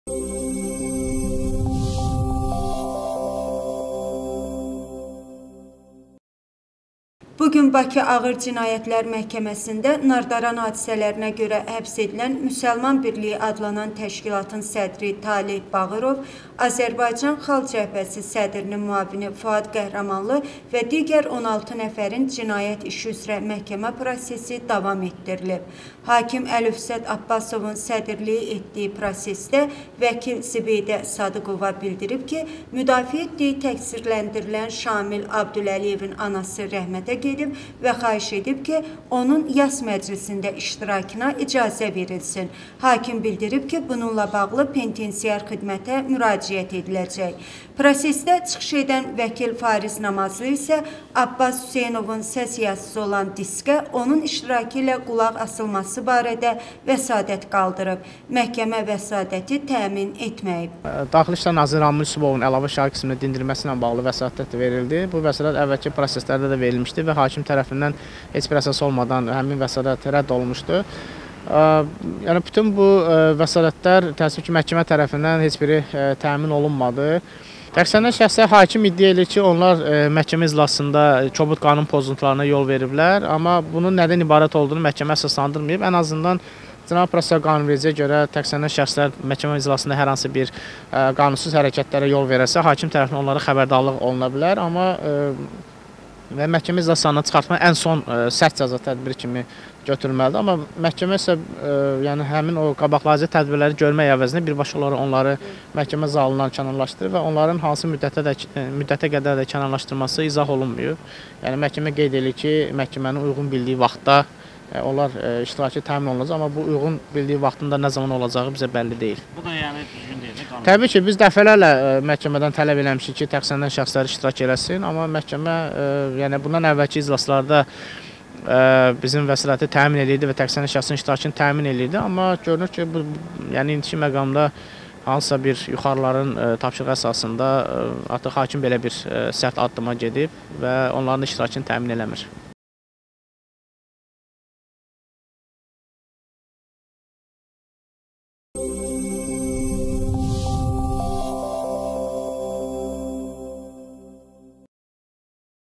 Nardaran hadisəsi ilə bağlı məhkəmə baş tutub
Bu gün Bakı Ağır Cinayətlər Məhkəməsində Nardaran hadisələrinə görə həbs edilən “Müsəlman birliyi” adlanan təşkilatın sədri Taleh Bağırov, Azərbaycan Xalq Cəbhəsi Partiyası (AXCP) sədrinin müavini Fuad Qəhrəmanlı və digər 16 nəfərin cinayət işi üzrə məhkəmə prosesi davam etdirilib.
Hakim Əlövsət Abbasovun sədrlik etdiyi prosesdə vəkil Zibeydə Sadıqova bildirib ki, müdafiə etdiyi təqsirləndirilən Şamil Abduləliyevin anası rəhmətə gedib və xahiş edib ki, onun yas məclisində iştirakına icazə verilsin. Hakim bildirib ki, bununla bağlı Penitensiar Xidmətə müraciət ediləcək.
Prosesdə çıxış edən vəkil Fariz Namazlı isə Abbas Hüseynovun səs yazısı olan diskə onun iştirakı ilə qulaq asılması barədə vəsatət qaldırıb. Məhkəmə vəsatəti təmin etməyib.